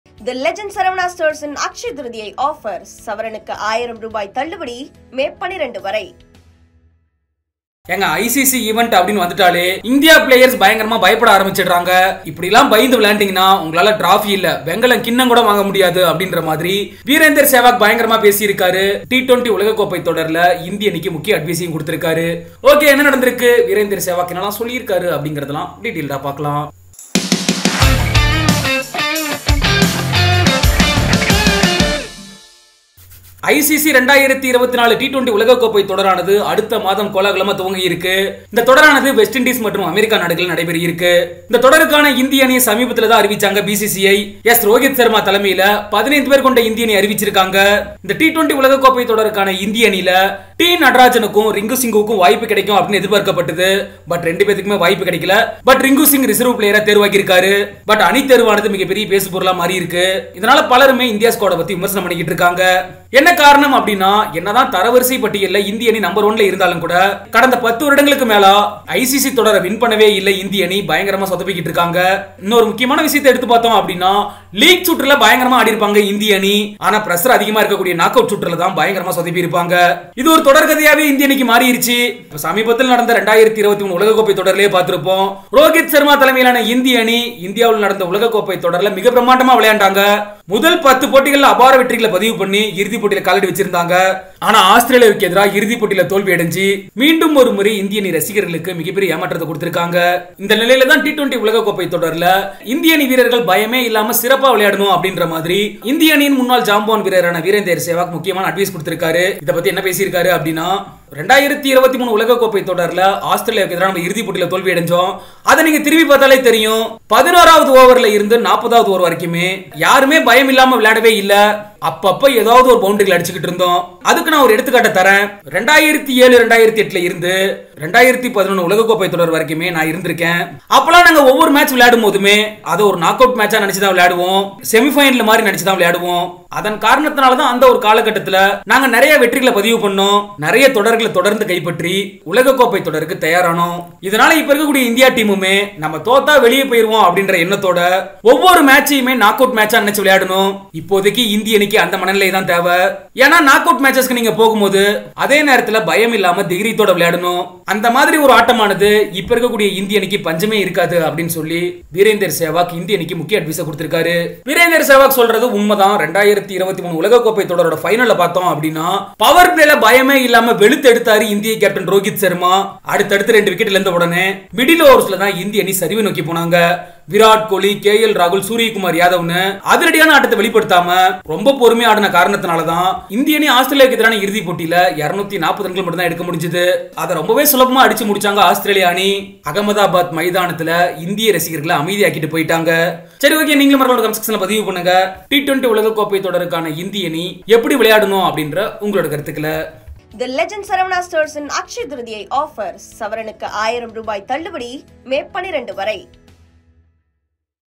T20 World Cup-ஐ India வெல்ல Fearless ஆட்டம் தேவை - Virender Sehwag | Oneindia Howzat
T20 World Cup 2024 Update | 'Lacks fearlessness and courage' - Virender Sehwag reveals what India needs to win ICC tournaments

       ~ED.71~HT.71~PR.57~